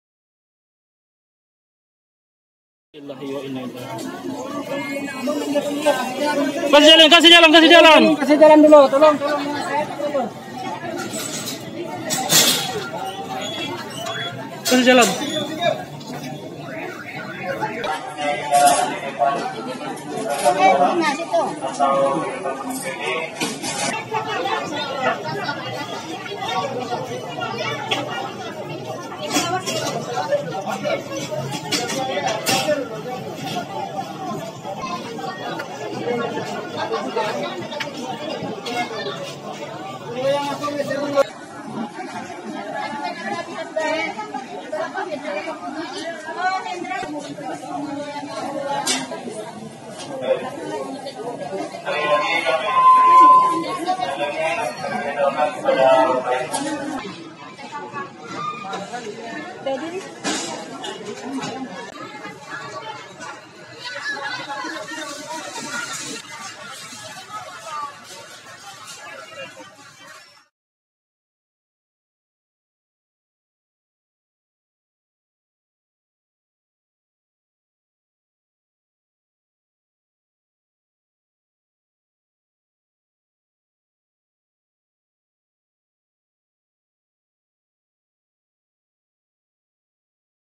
Scenes at hospital after deadly quake strikes Indonesia's Sumatra Island | The Rutendo
Injured people are brought to a hospital after a deadly 6.2-magnitude earthquake struck Indonesia's Sumatra island.